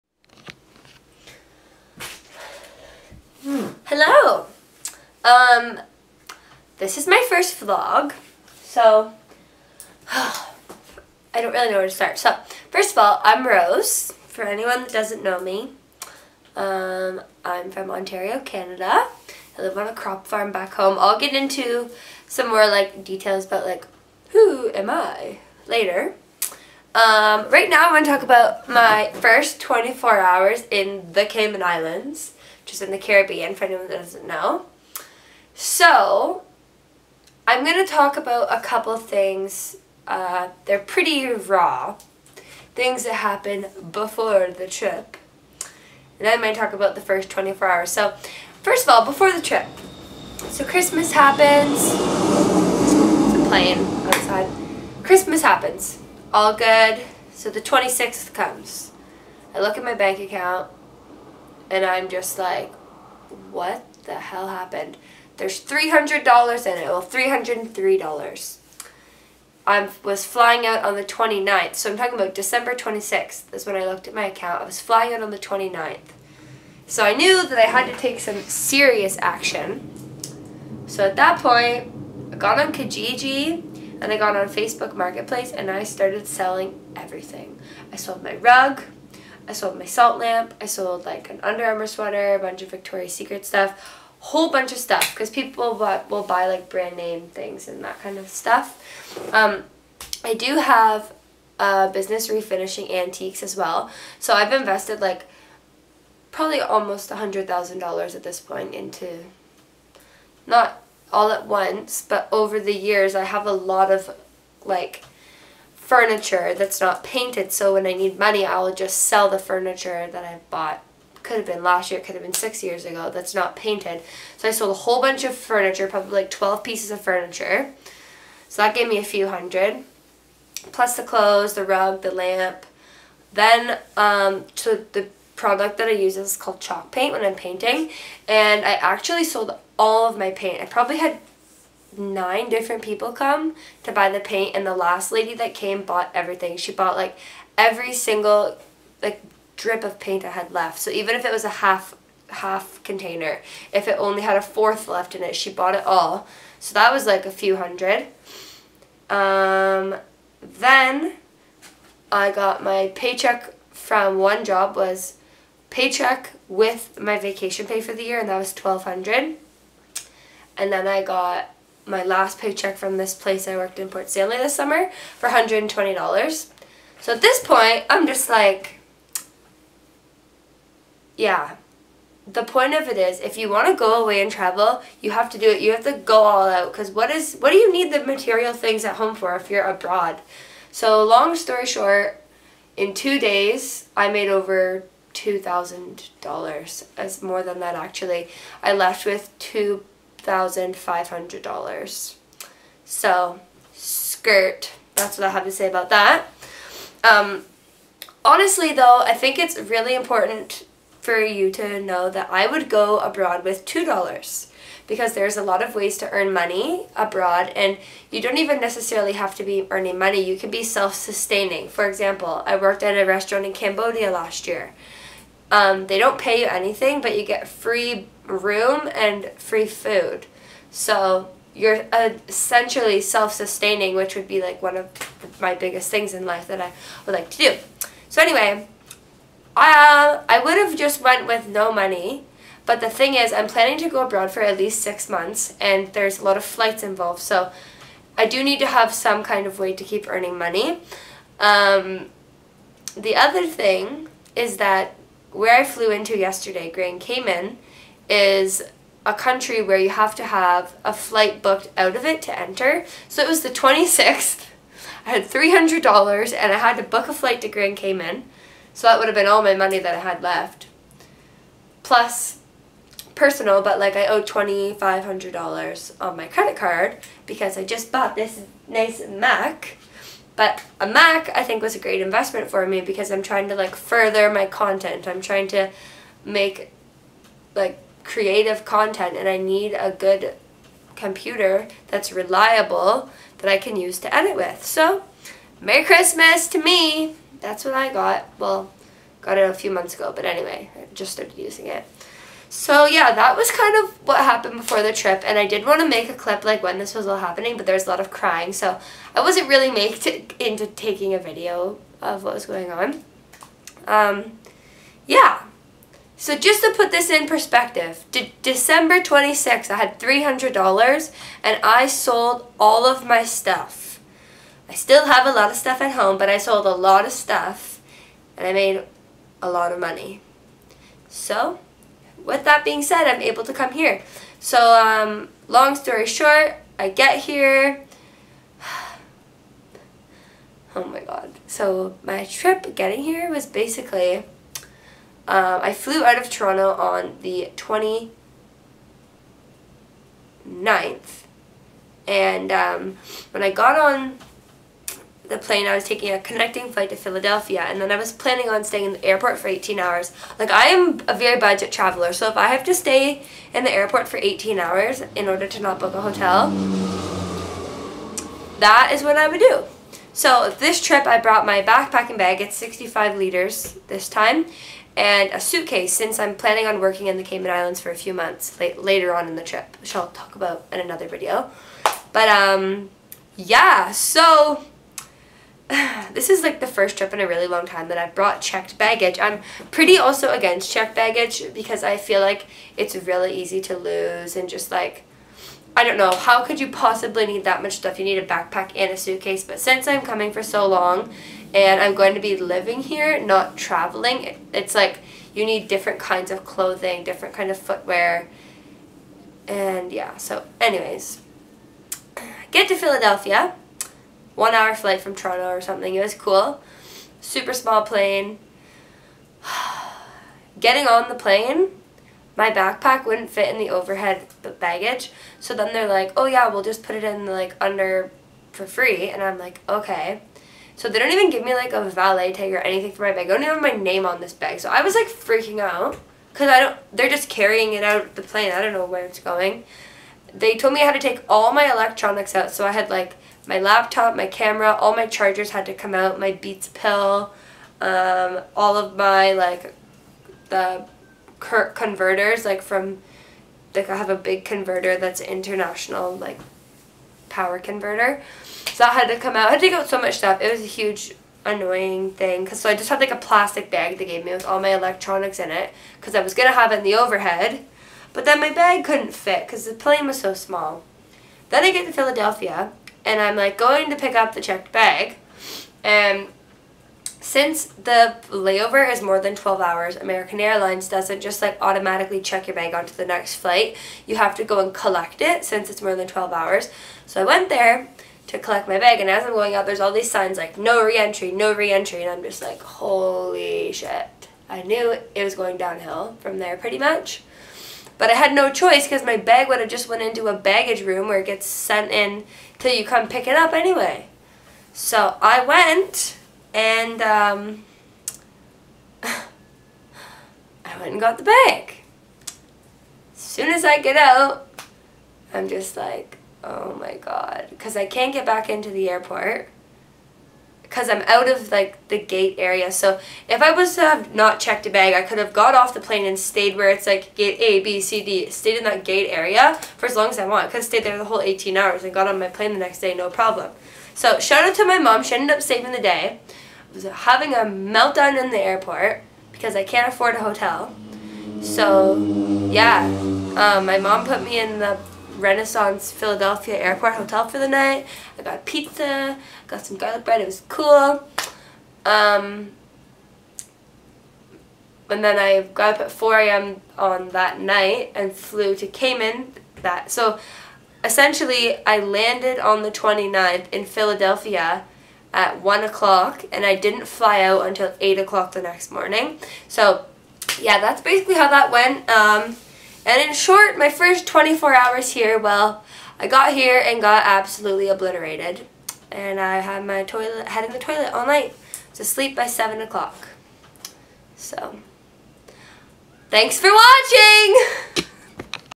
How I sold everything to move to the Cayman Islands ( before the trip, travel)
Hello!! This is my first vlog. Yes I know its not in focus but thats okay bc its my first one.